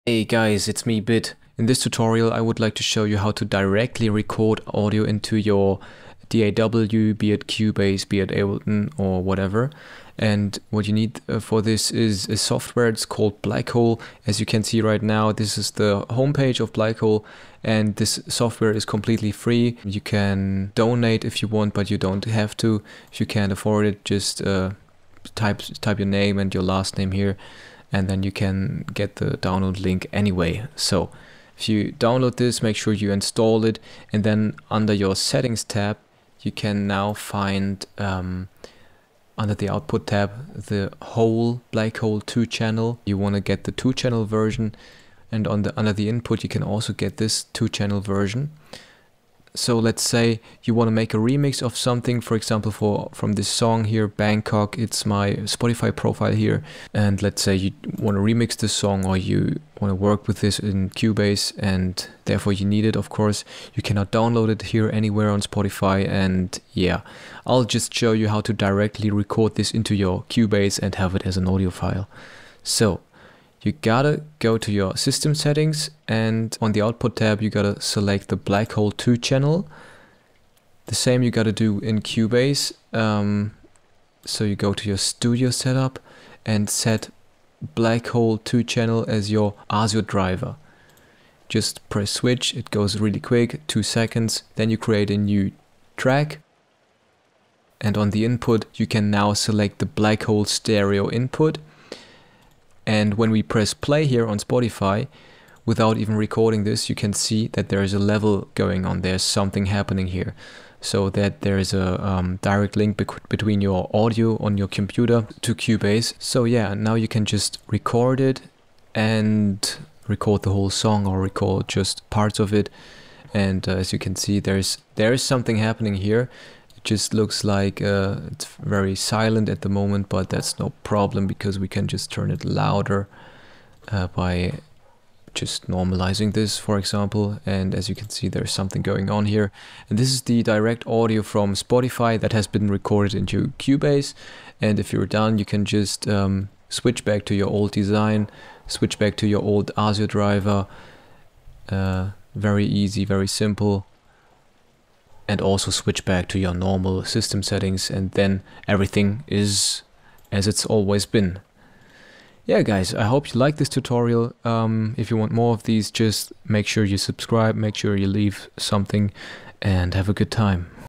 How to Record Desktop Audio DIRECTLY into your DAW | MAC ONLY | Cubase Tutorial
In this video you will learn how to directly record Audio from your desktop into Cubase or any other DAW that you are using. Should work with the most programs.

If you have trouble or can't find BlackHole as an input device when creating a new audiotrack, press F4 and make sure cubase accepted Blackhole as your new audio device.

On this channel you will find synthwave/piano covers/and other stuff based around music.